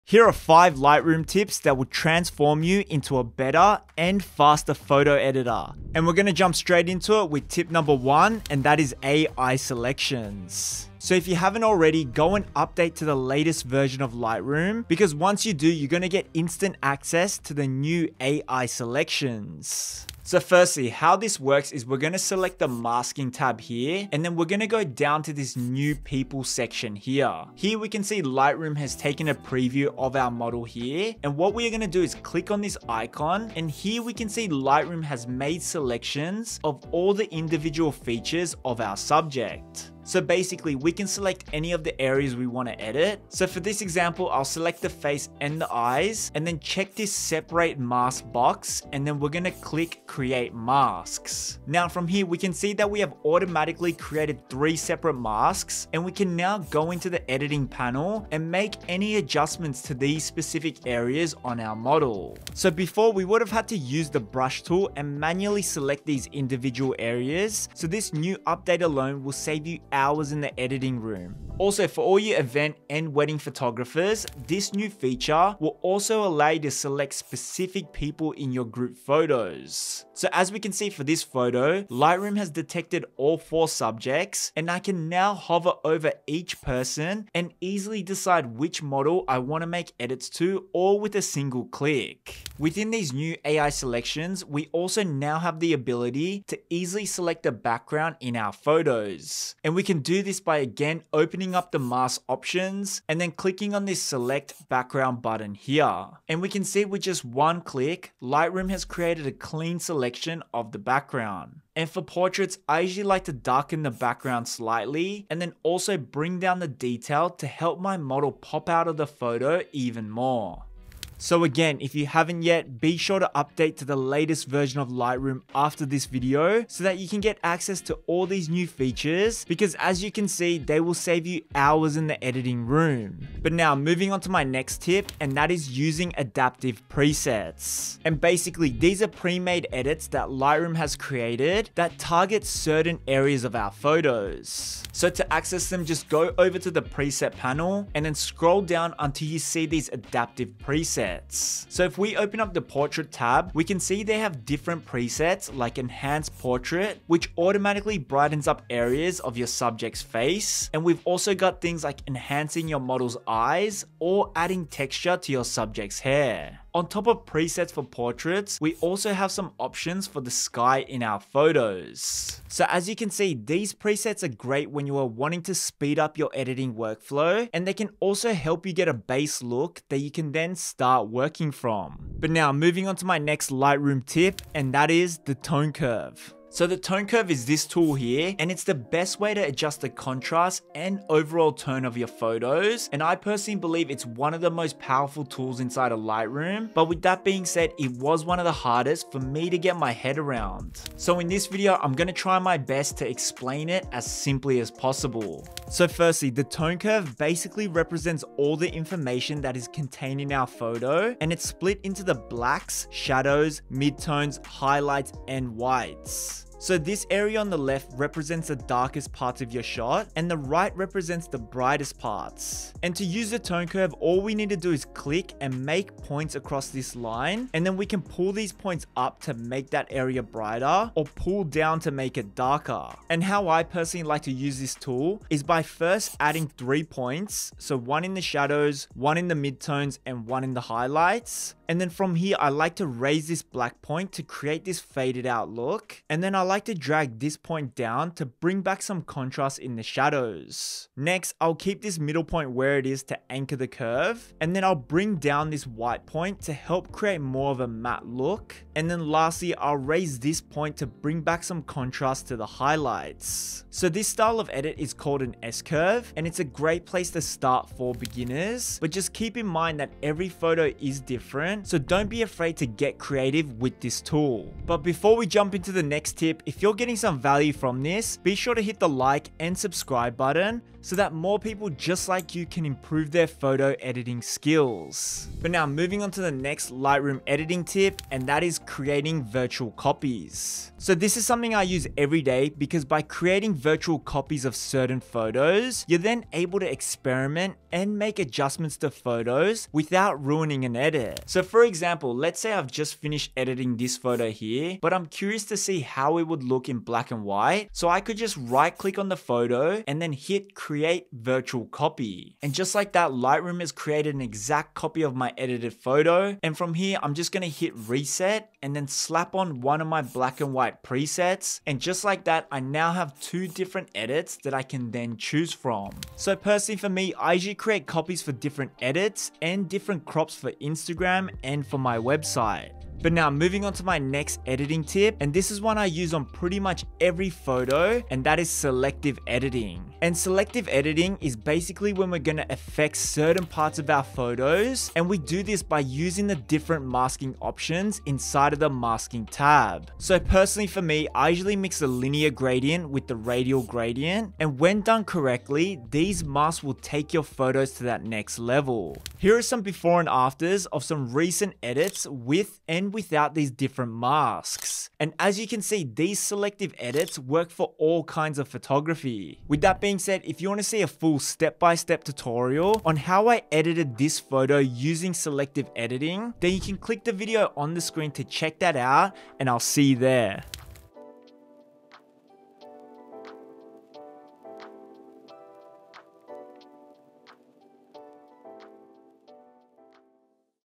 5 LIGHTROOM TIPS To TRANSFORM Your PHOTOS In 2023!
In this video you will discover 5 Adobe Lightroom tips that will help you streamline your editing workflow, and give you the confidence to edit better photos!

These editing techniques work on all versions of Lightroom, so whether you're using Adobe Lightroom Classic, Lightroom Desktop, or Lightroom Mobile, these tips will help you become a better photo editor!

Time Stamps:
0:00 Intro
0:06 Tip 1
2:13 Tip 2
3:02 Tip 3
4:45 Tip 4
5:32 Tip 5

🎨 All photos seen in this video were editing using my personal Preset packs!